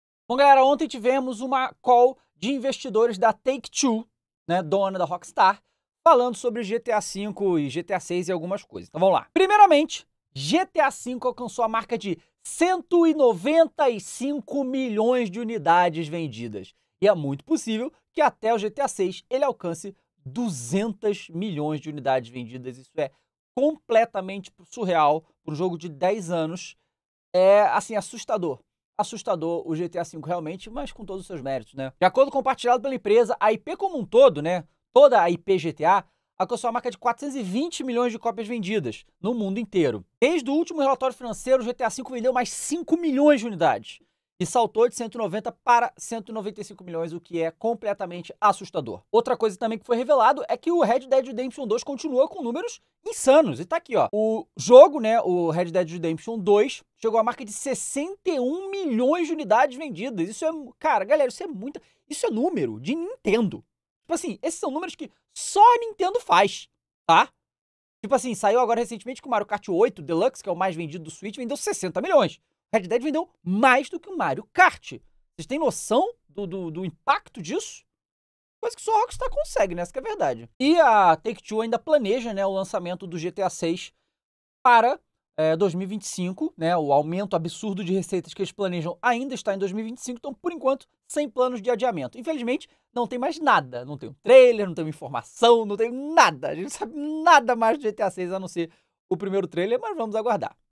Você NÃO VAI ACREDITAR no que o GTA V conseguiu depois de 10 ANOS
Fala galera! No vídeo de hoje teremos um corte comentando sobre uma notícia de GTA V.